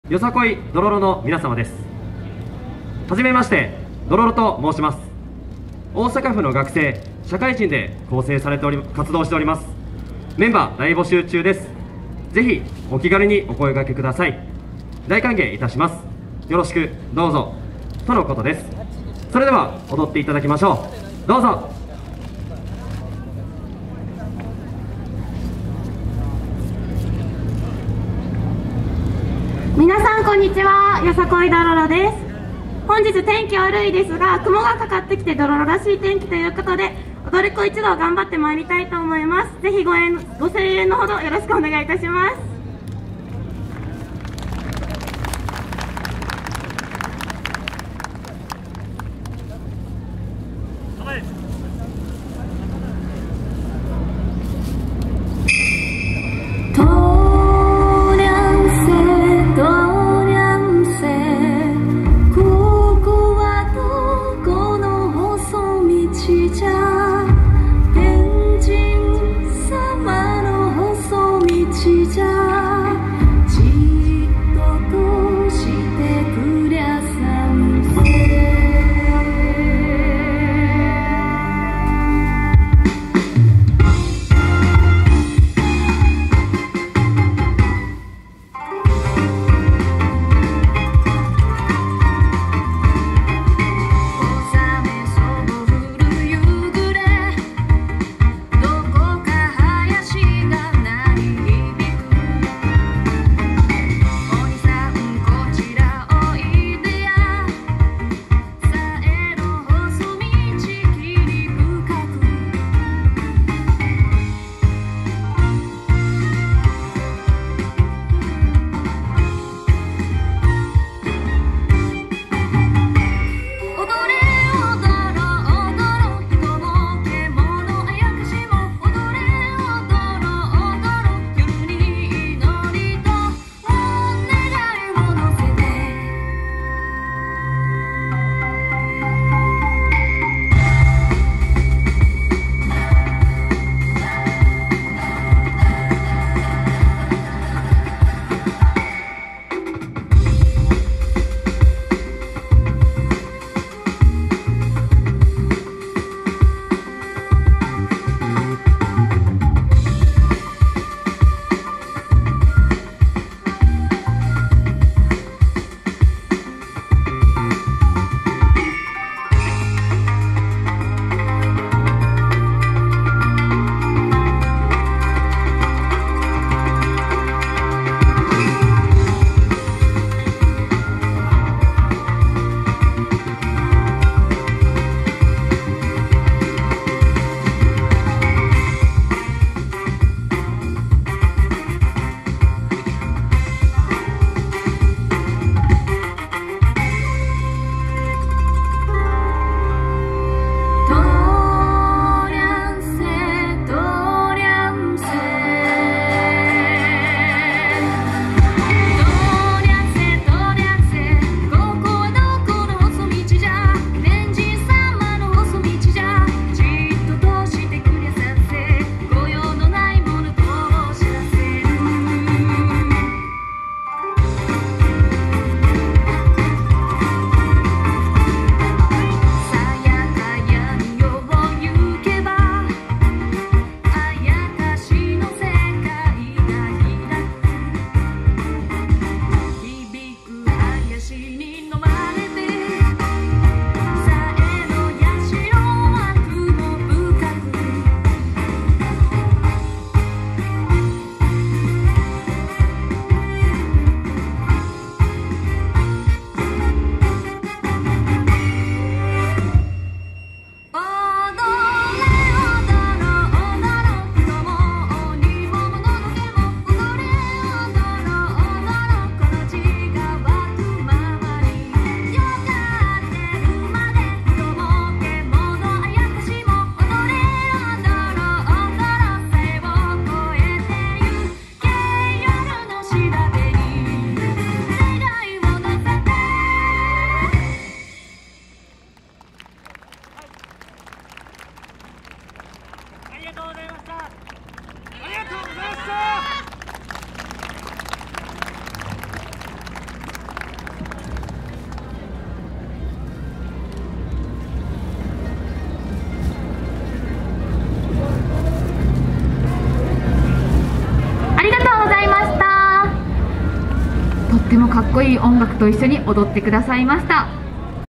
[4K] よさこいどろろ こいや祭り 2021 ２日目
※公式カメラマンとして撮影しています。
2021年10月31日(日)
舞洲スポーツアイランド　空の広場メインステージ